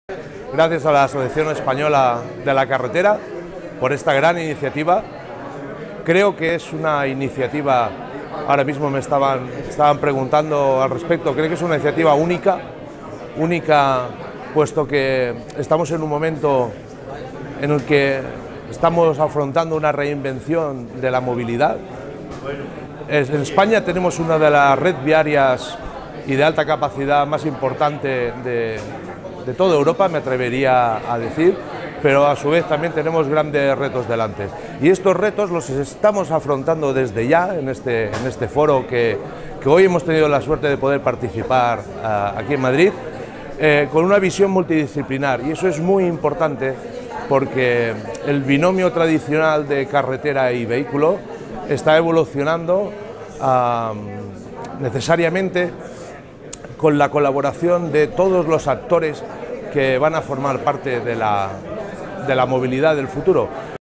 Marius Gómez (T-Systems) | Jornada de lanzamiento Foro Español de Smart Roads
Marius Gómez
T-Systems
“Estamos afrontando una reinvención de la movilidad en la que la visión multidisciplinar que aporta el Foro Español de Smart Roads es fundamental”